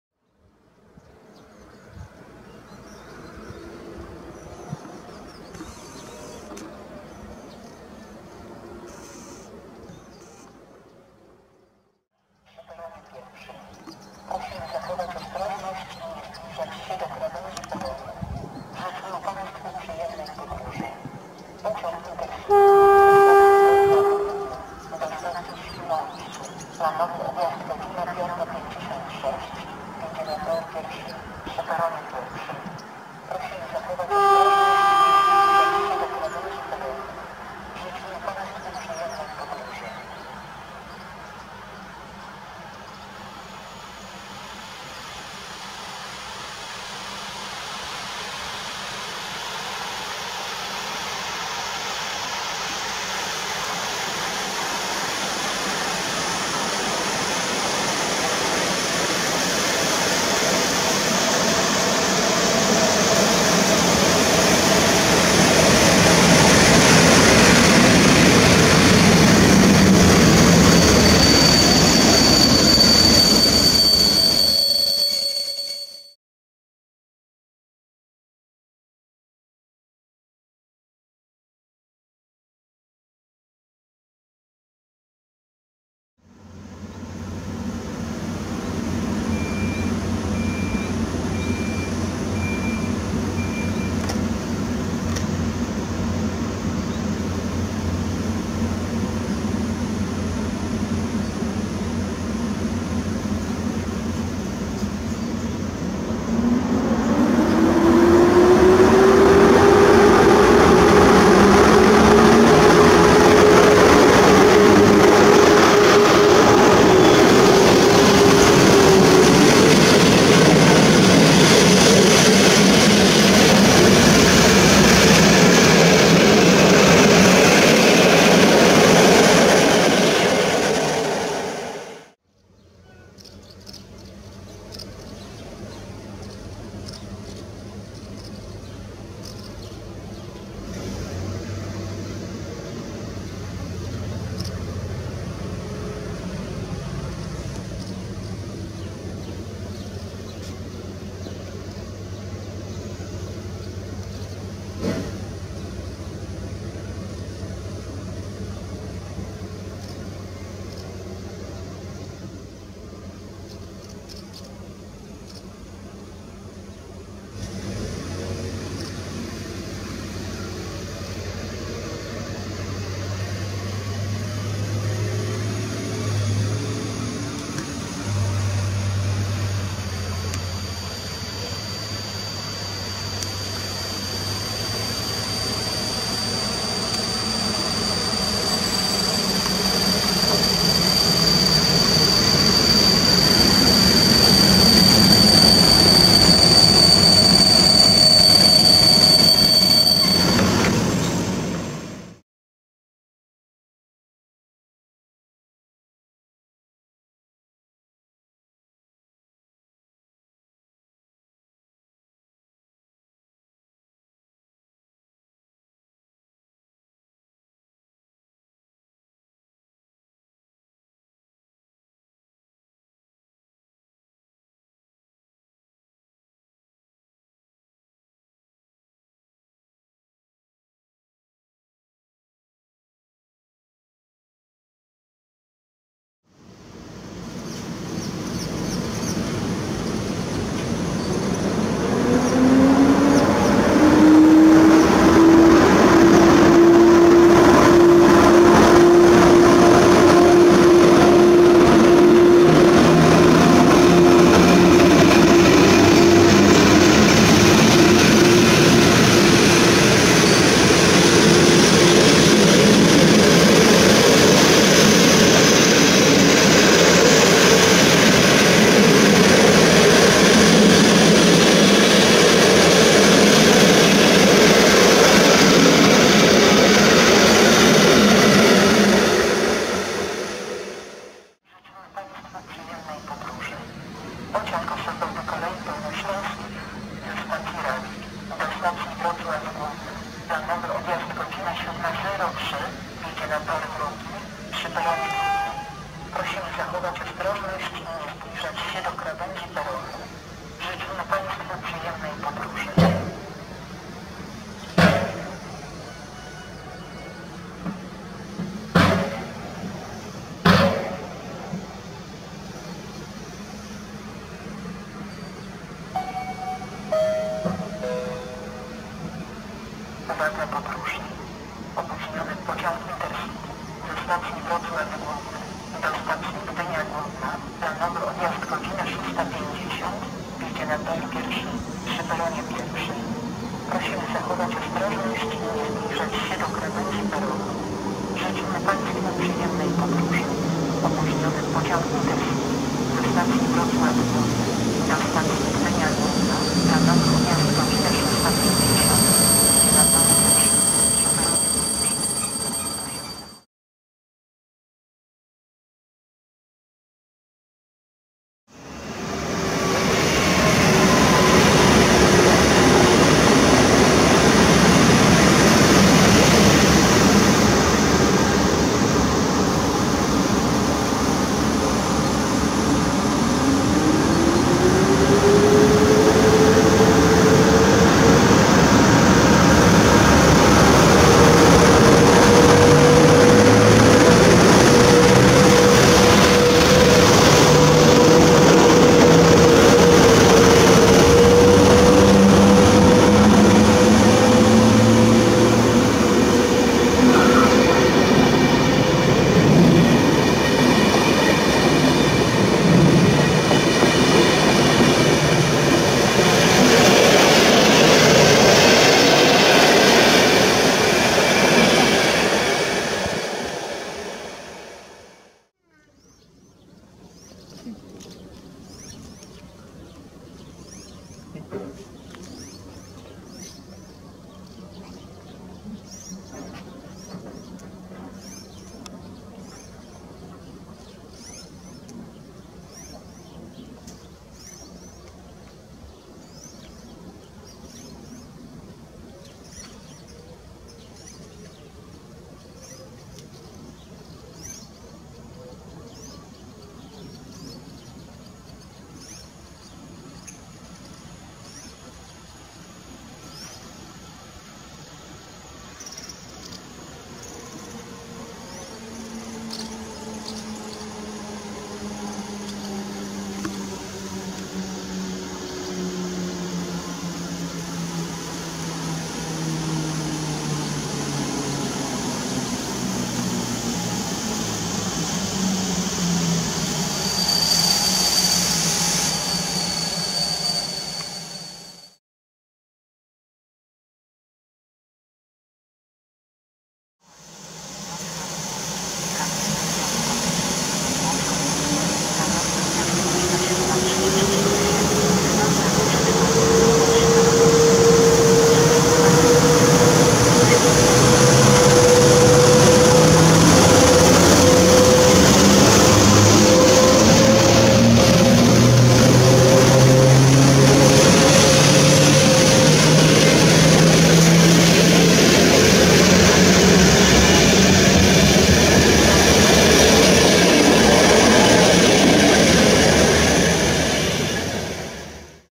Poranne pociągi PKP Intercity na stacji Żmigród. +RP1 i Zapowiedzi
Cztery składy pociągów PKP Intercity: Bryza,Astra,Mieszko i Siemiradzki o wschodzie słońca na stacji Żmigród.

Spis Pociągów:
1. EP07-335 z IC 68152/3 Bryza do Świnoujścia. +RP1
2. EP07-1051 z TLK 56260/1 Astra do Wrocławia Głównego.
3. EP09-015 z IC 6508/9 Mieszko do Gdyni Głównej +RP1 i wjazd 31WE-004 jako Os 69624 do Wrocławia Głównego.
4. EP09-030 z IC 7302/3 Siemiradzki do Przemyśla Głównego.

Województwo: Dolnośląskie
Miejscowość: Żmigród
Data: 29 Lipiec 2016r.
Czas: 05:53-07:35
Przewoźnik: PKP Intercity
D29: 271 (Wrocław-Poznań)
Montaż: Camtasia Studio